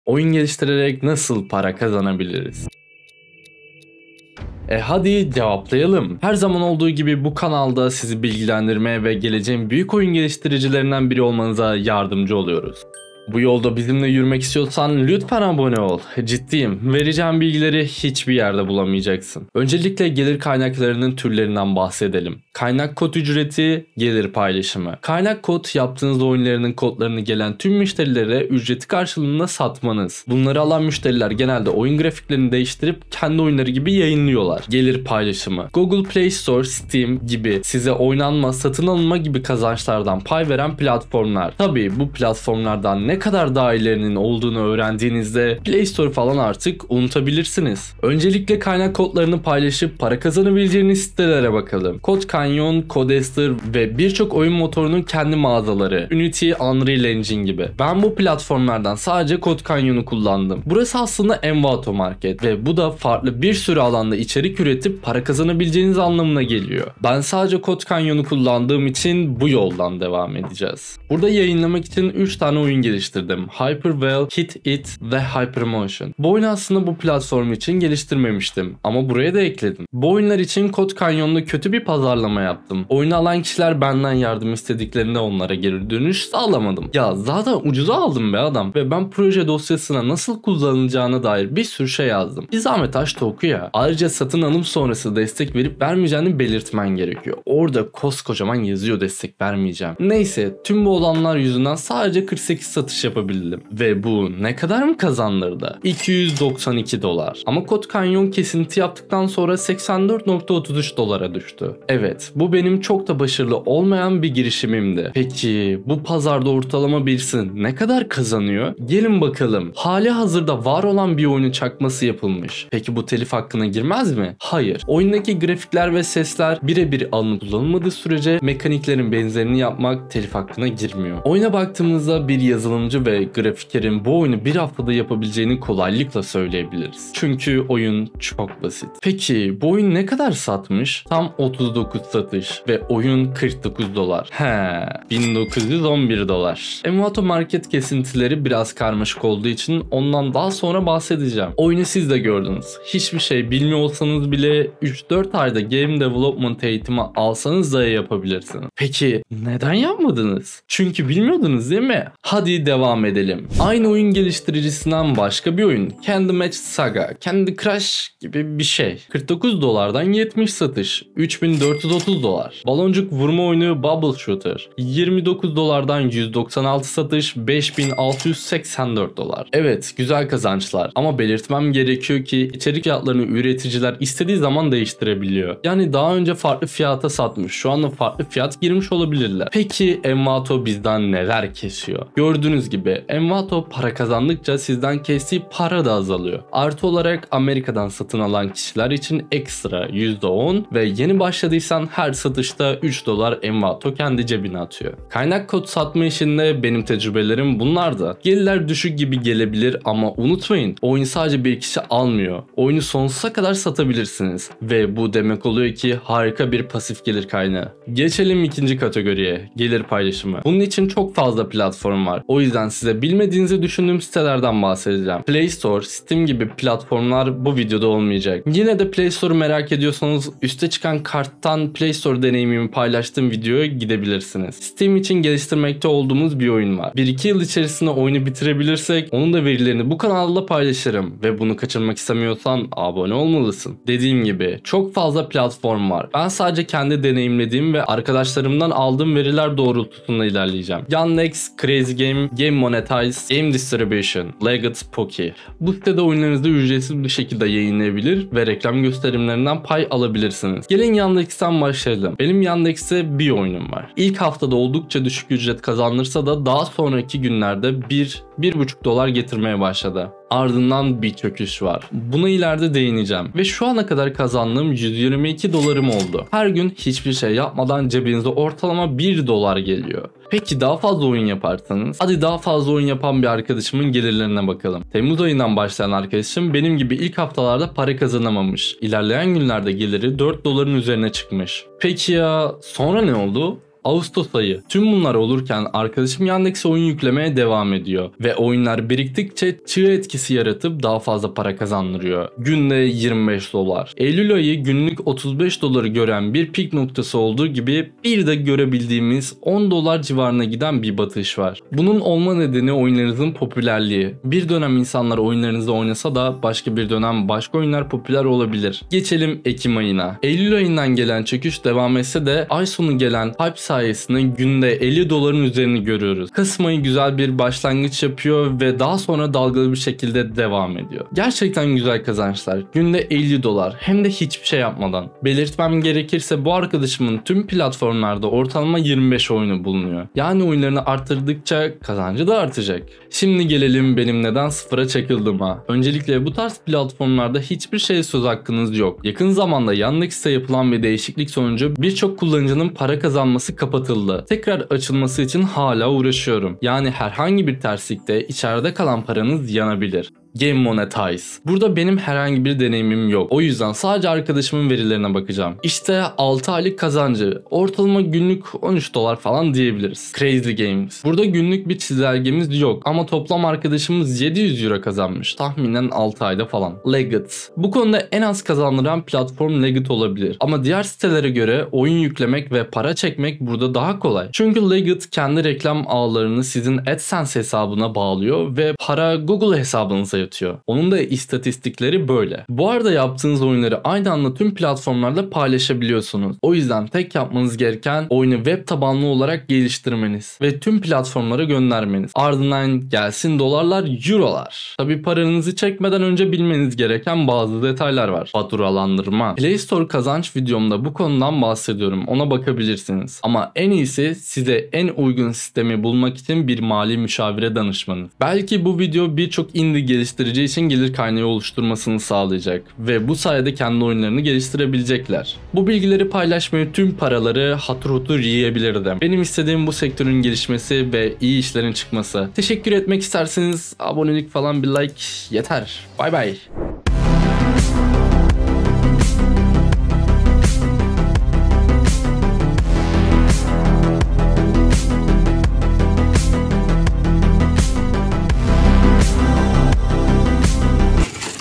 Oyun Yaparak Para Kazan
Oyun geliştirerek nasıl para kazanılır sorusunu cevapladığımız ve oyunlardan ne kadar para kazandım onu açıklıyorum. Oyun geliştirme ne kadar kazandırıyor bunu gösteriyorum

Oyunumuzu İstek Listesine Alırsan Süper Olur knk.